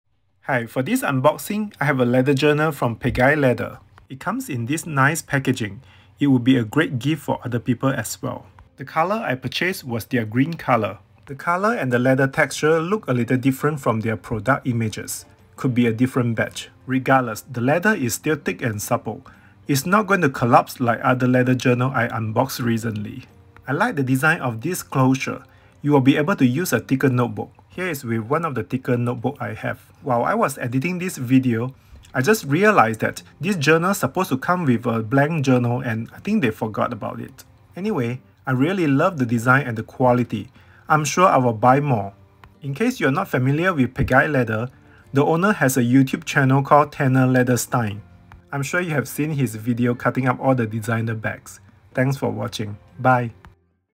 Pegai Leather Unboxing & First Impression Review | DuPage Leather Journal
Hi, I had a leather journal from Pegai Leather(@pegai8003) for this unboxing and first impression review video. It's their DuPage Leather Journal in green color. If you are unfamiliar with Pegai Leather Goods, the owner has a YouTube Channel called @tanner.leatherstein, one of my favorites on YouTube. I love the design of this notebook cover. With this closure design, you will be able to use thicker notebooks. Their leather journals come in 3 sizes, so if you are into journaling, you will be able to find one you like!